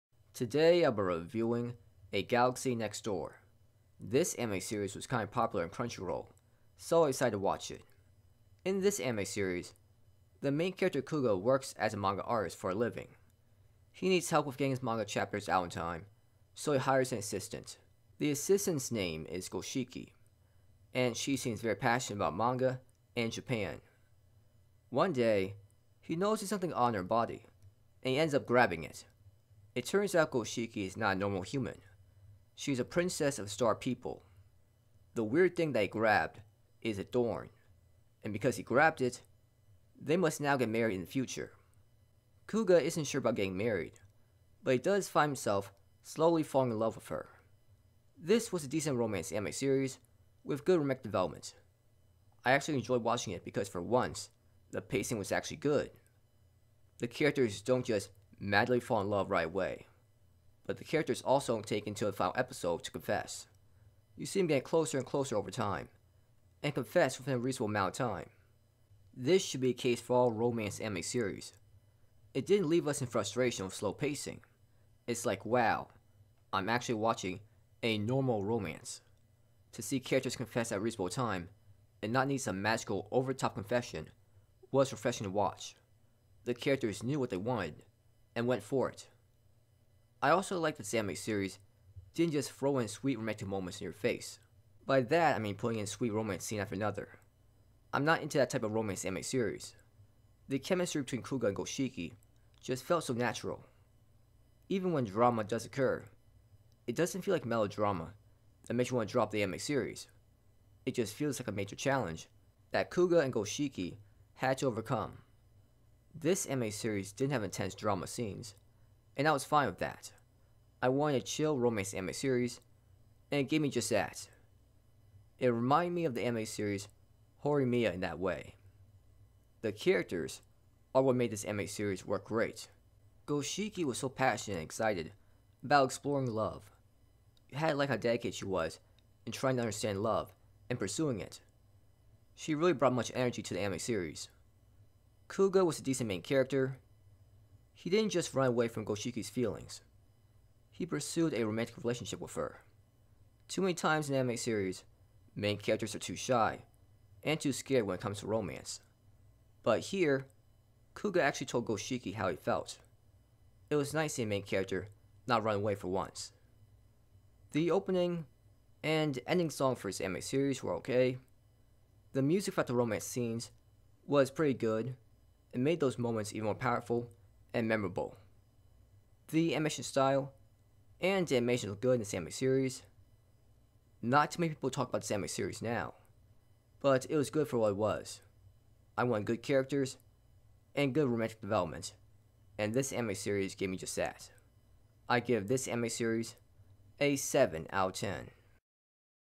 A Galaxy Next Door Anime Review, A Good Romance Anime With Great Romantic Development
A Galaxy Next Door Anime Review